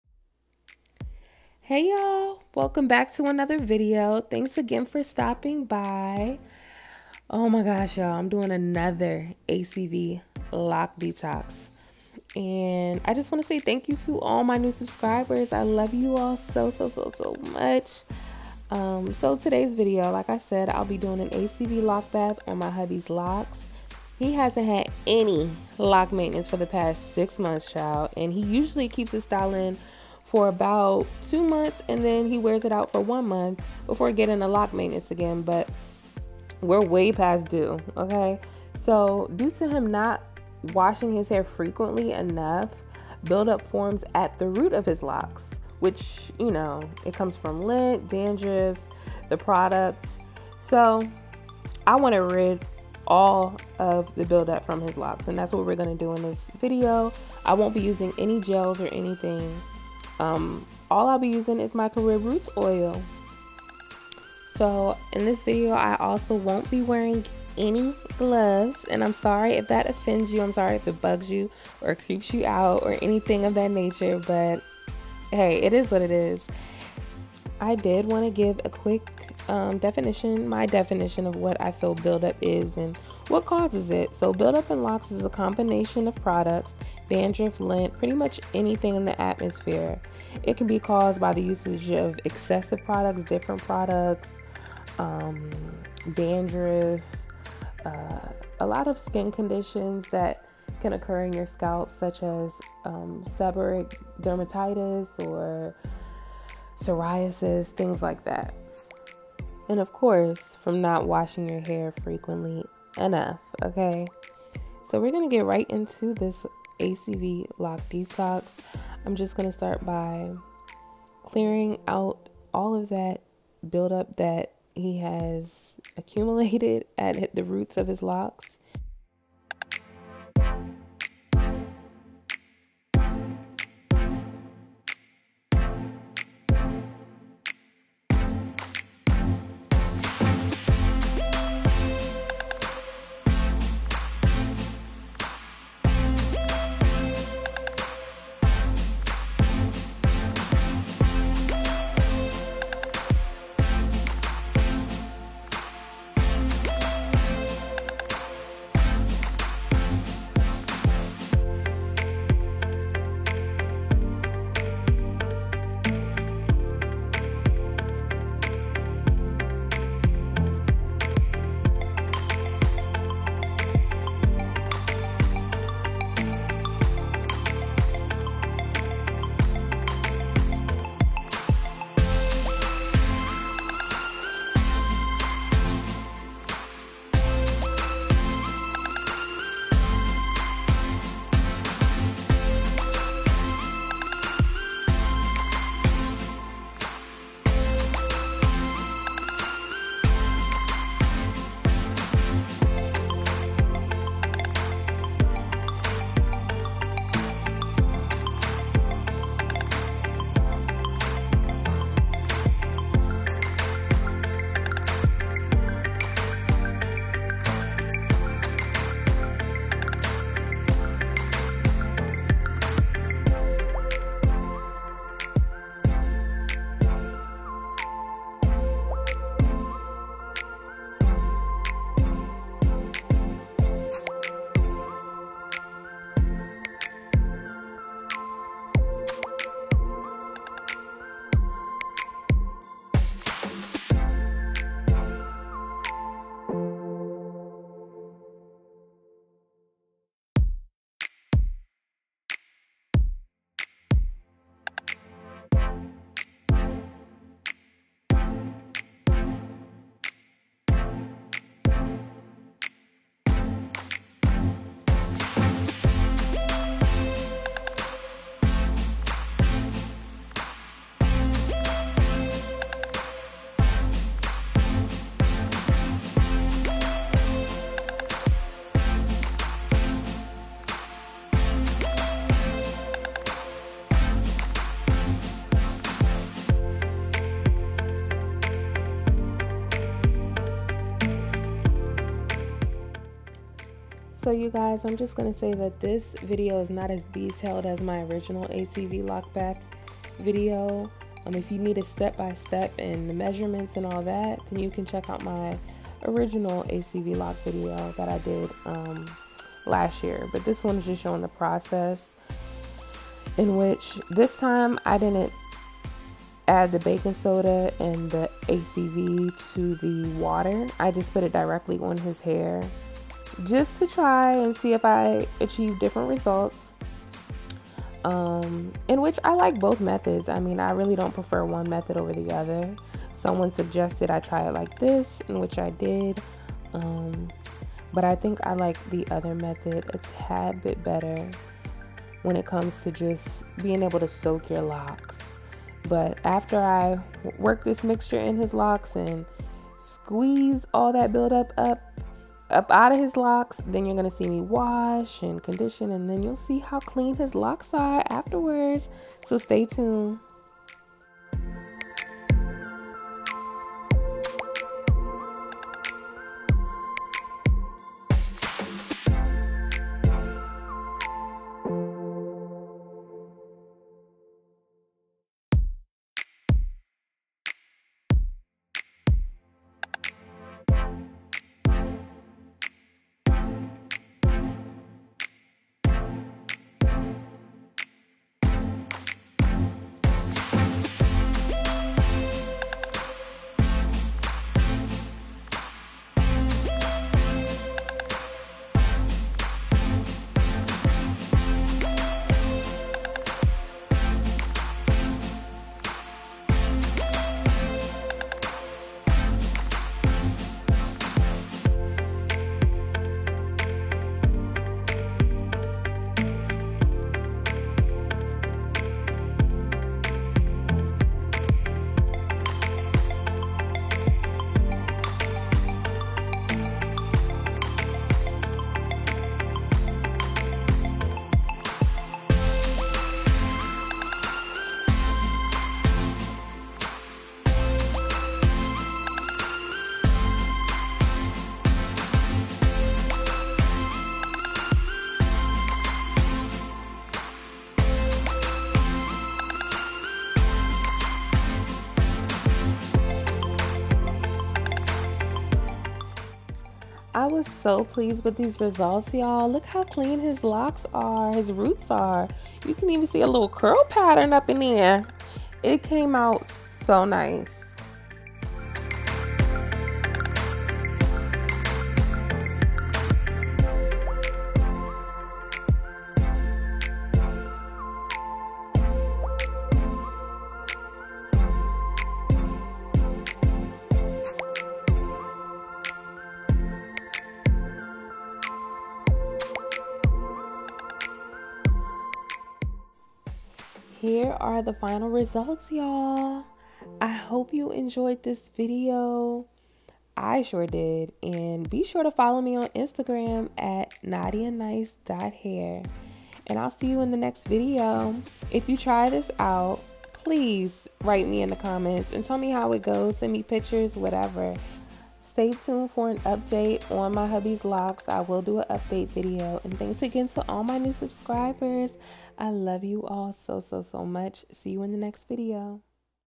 INTENSIVE ACV LOC DETOX | NO GEL, ONLY OIL USED TO RETWIST
Hey y'all! In today's video, I demonstrate how I got rid of all the buildup in my hubby's locs again on Valentine's Day. I didn't use any gel to retwist his locs, only my handmade oil. Love y'all, and enjoy!